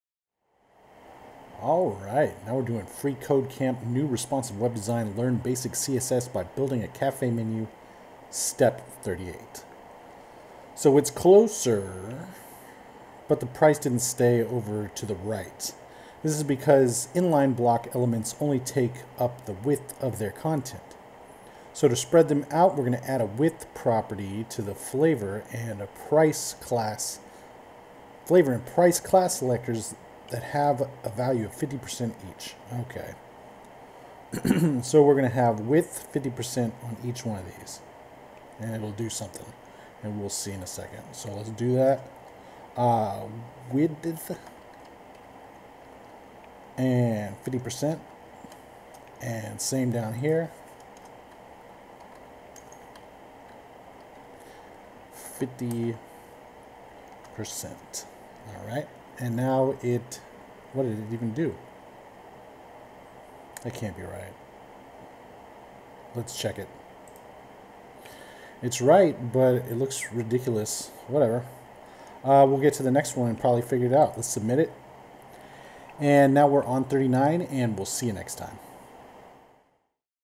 Learn Basic CSS by Building a Cafe Menu - Step 38
this one shows us how to make some space with the width property